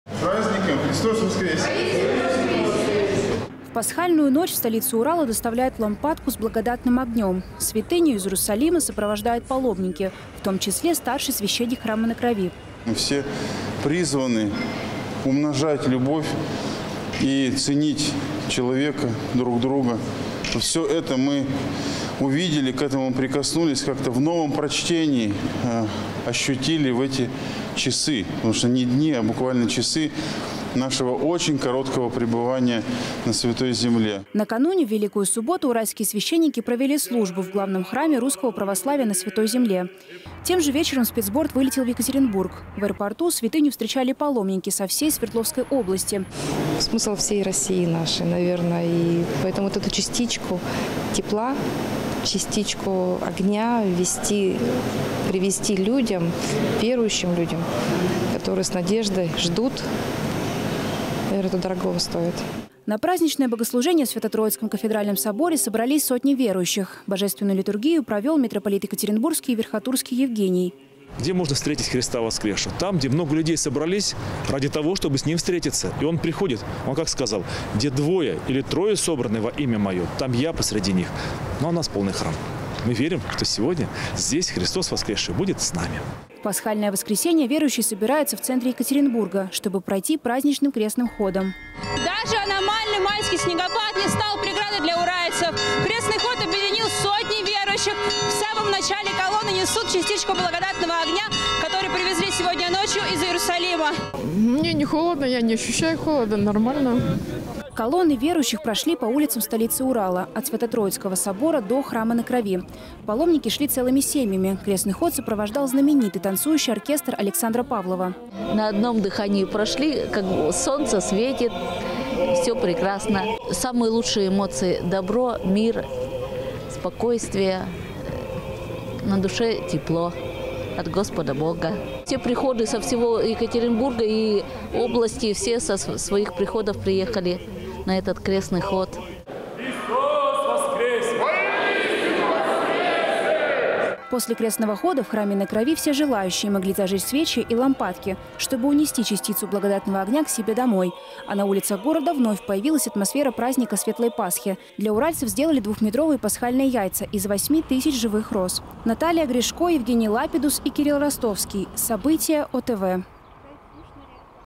Благодатный огонь и танцующий оркестр: Пасхальные чудеса в столице Урала
Благодатный огонь со Святой Земли и двухметровые яйца из живых цветов. В Свердловской области тысячи верующих отметили один из главных православных праздников – Пасху. Настроение уральцам не омрачил даже майский снегопад. В Пасхальную ночь в столицу Урала доставляют лампадку с Благодатным огнём. Святыню из Иерусалима сопровождают паломники, в том числе старший священник Храма на Крови. Максим Миняйло: «Мы все призваны умножать любовь и ценить человека, друг друга. Всё это мы увидели, к этому прикоснулись как-то в новом прочтении, ощутили в эти часы, потому что не дни, а буквально часы нашего очень короткого пребывания на Святой Земле». Накануне в Великую субботу уральские священники провели службу в главном храме русского православия на Святой Земле. Тем же вечером спецборт вылетел в Екатеринбург. В аэропорту святыню встречали паломники со всей Свердловской области. Галина Баланюк специально приехала из Алапаевска. Галина Баланюк: «Смысл всей России нашей, наверное, и поэтому, вот эту частичку тепла, частичку огня привезти людям, верующим людям, которые с надеждой ждут, это дорогого стоит». На праздничное богослужение в Свято-Троицком кафедральном соборе собрались сотни верующих. Божественную литургию провел митрополит Екатеринбургский и Верхотурский Евгений. Митрополит Евгений: «Где можно встретить Христа воскресшего, там, где много людей собрались, ради того, чтобы с ним встретиться. И он приходит, он как сказал: где двое или трое собраны во имя моё, там я посреди них, ну а нас полный храм. Мы верим, что сегодня Христос воскресший будет с нами». В Пасхальное воскресенье верующие собираются в центре Екатеринбурга, чтобы пройти праздничным крестным ходом. Даже аномальный майский снегопад не стал преградой для уральцев. Крестный ход объединил сотни верующих. В самом начале колонны несут частичку благодатного огня, который привезли сегодня ночью из Иерусалима. Колонны верующих прошли по улицам столицы Урала от Свято-Троицкого собора до Храма на Крови. Паломники шли целыми семьями. Крестный ход сопровождал знаменитый танцующий оркестр Александра Павлова. Вера Смирнова: «На одном дыхании прошли, солнце, всё прекрасно. Самые лучшие эмоции, добро, мир, спокойствие, на душе тепло от Господа Бога. Все приходы со всего Екатеринбурга и области. Все со своих приходов приехали на это крестный ход». После крестного хода в Храме на Крови все желающие могли зажечь свечи и лампадки, чтобы унести частицу благодатного огня к себе домой. А на улицах города вновь появилась атмосфера праздника Светлой Пасхи. Для уральцев сделали двухметровые пасхальные яйца из 8 тысяч живых роз.